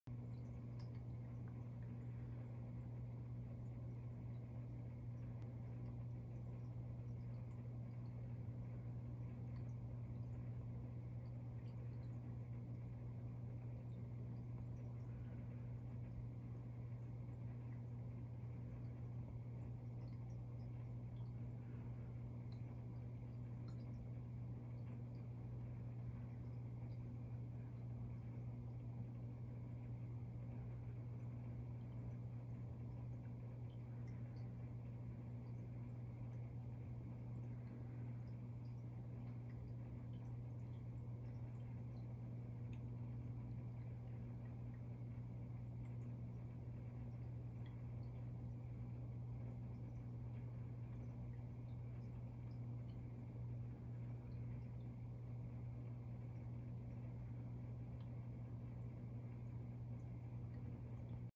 Black and White Ocellaris Clownfish & Bluehead Wrasse
Black and white Ocellaris clownfish swimming through the night lighting of the 5 gallon saltwater aquarium.